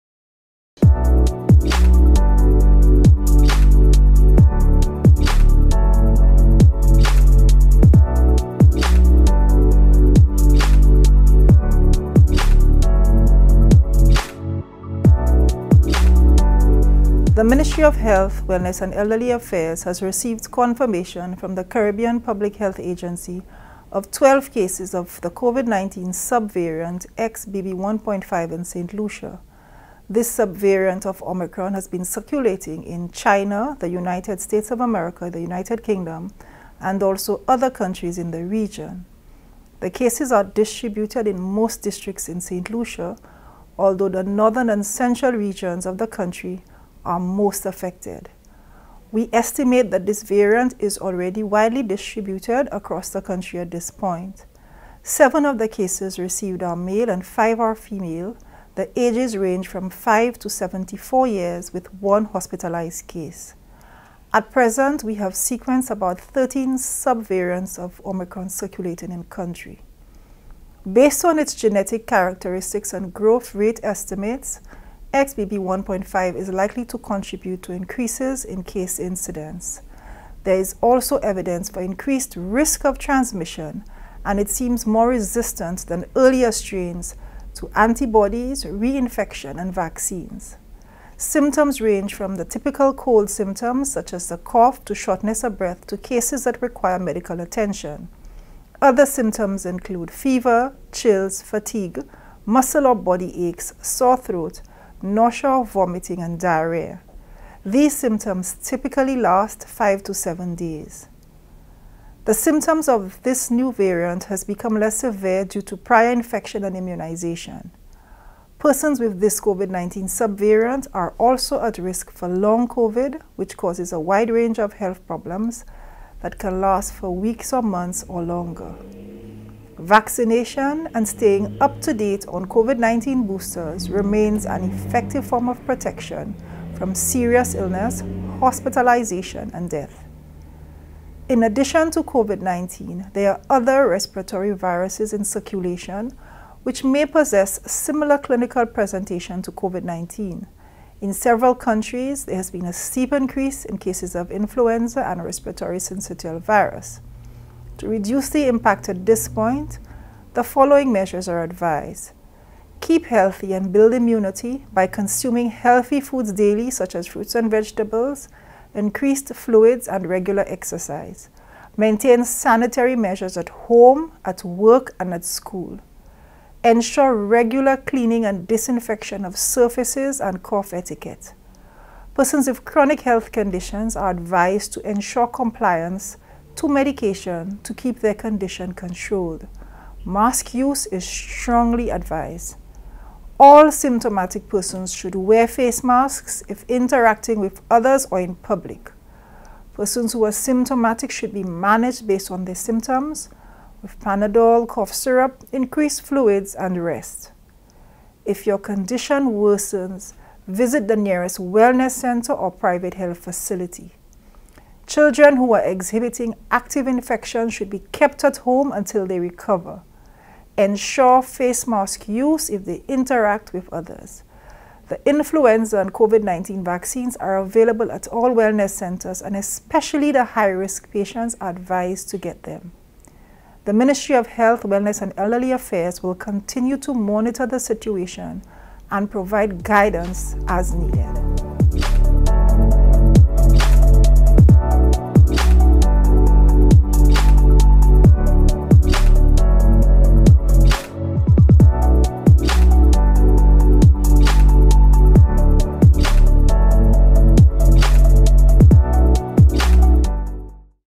Statement Public Health Update February 16, 2023
The Ministry of Health, Wellness and Elderly Affairs has received confirmation from the Caribbean Public Health Agency of 12 cases of COVID-19 sub variant XBB1.5 in Saint Lucia. This sub variant of Omicron has been circulating in China, the United States, United Kingdom and also other countries in the region. The cases are distributed in most districts in Saint Lucia, although the northern and central regions of the country are most affected. We estimate that this variant is already widely distributed across the country at this point. 7 of the cases received are male and 5 are female, ages range from 5-74 years with one hospitalized case. At present we have sequenced about 13 sub-variants of omicron circulating in country.